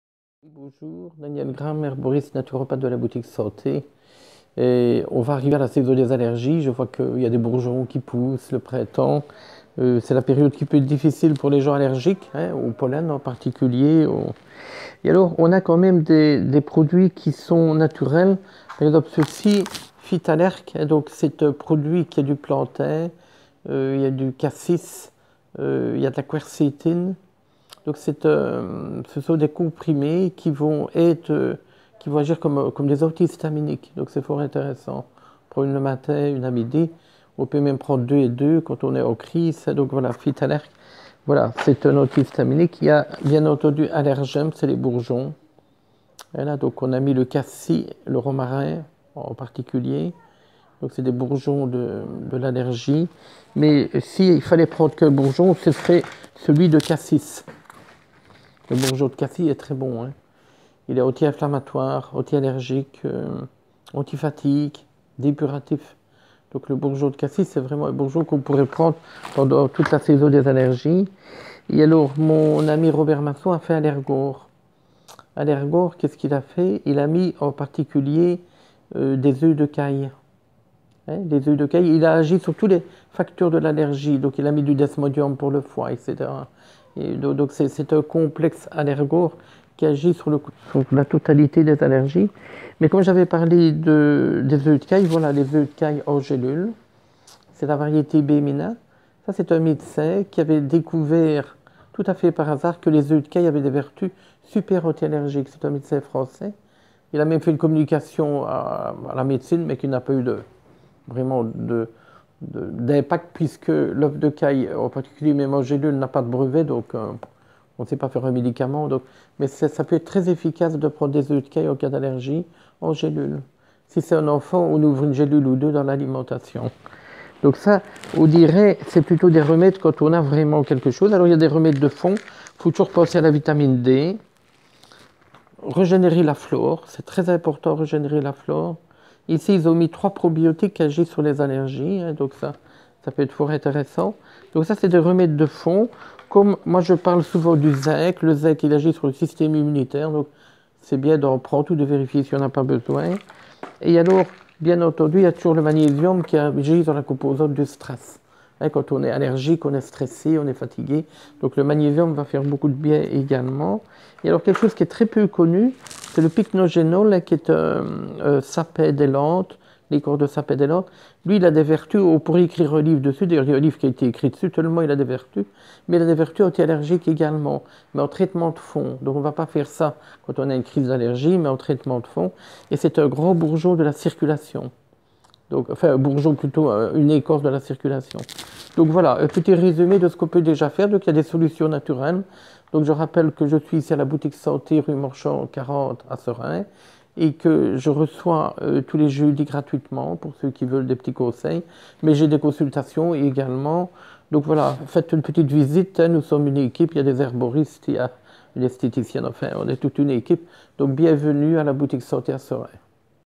Remèdes naturels contre les allergies
Il existe des solutions naturelles et efficaces, sous forme de compléments alimentaires et de plantes qui permettent de renforcer le système immunitaire et de rééquilibrer le terrain allergique